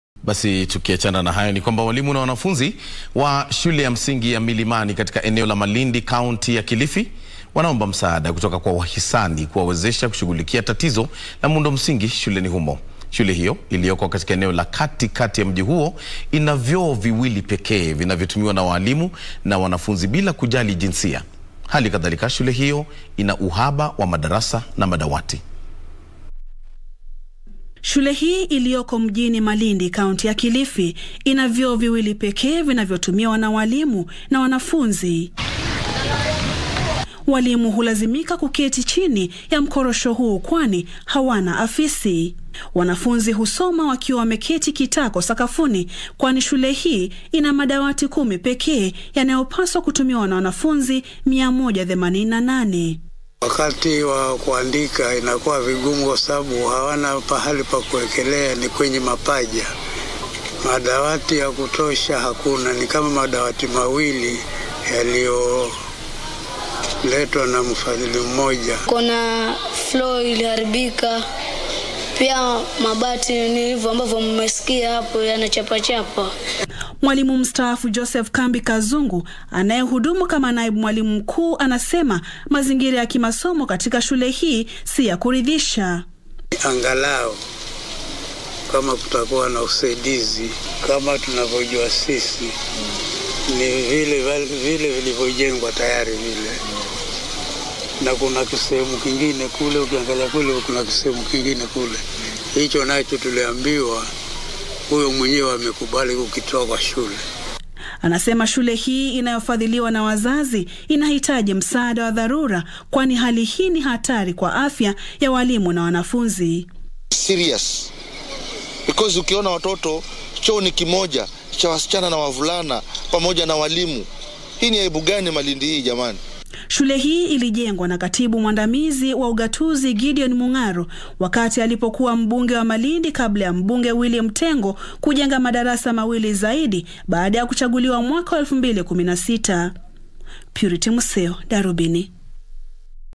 Wanafunzi waomba msaada baada ya mti wa korosho kugeuzwa kuwa darasa Malindi
Walimu na wanafunzi wa shule ya msingi ya Milimani katika eneo la Malindi, kaunti ya Kilifi wanaomba msaada kutoka kwa wahisani kuwawezesha kushughulikia tatizo la muundo msingi shuleni humo. Shule hiyo iliyoko katika eneo la kati kati ya mji huo ina vyoo viwili pekee vinavyotumiwa na walimu na wanafunzi bila kujali jinsia. Hali kadhalika shule hiyo ina uhaba wa madarasa na madawati.